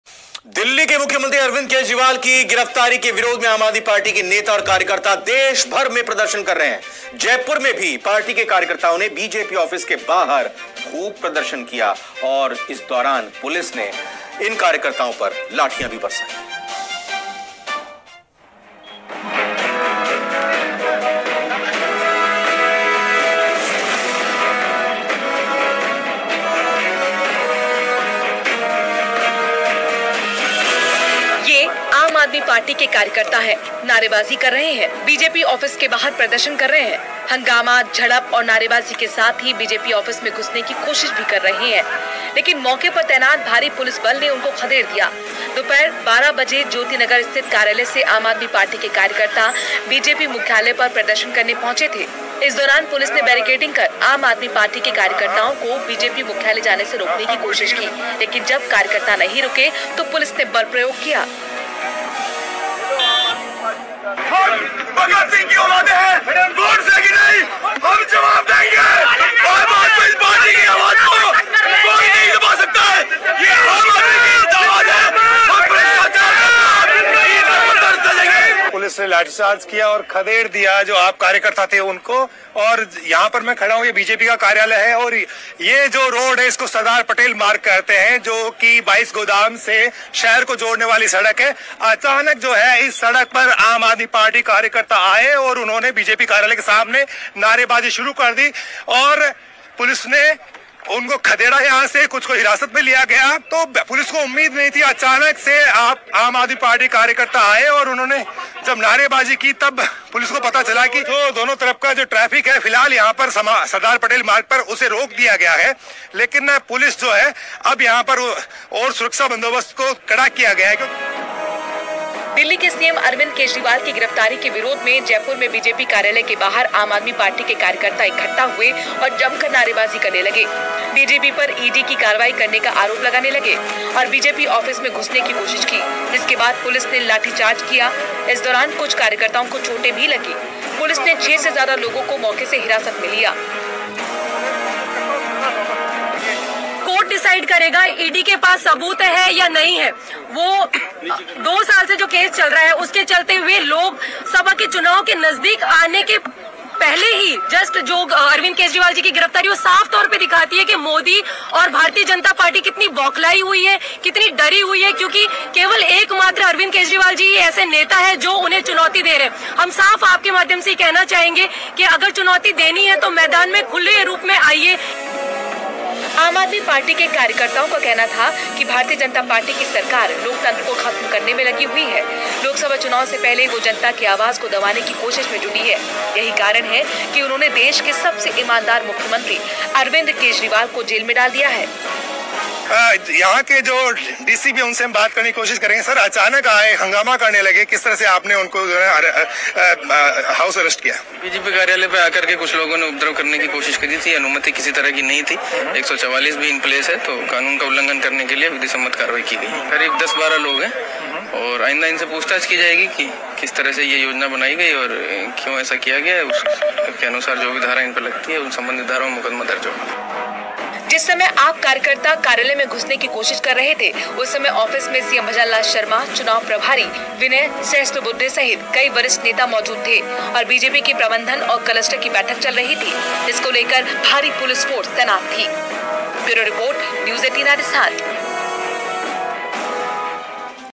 केजरीवाल की गिरफ्तारी। आम आदमी पार्टी का बीजेपी कार्यालय पर जमकर प्रदर्शन,पुलिस का लाठीचार्ज
Video from Bl Dhayal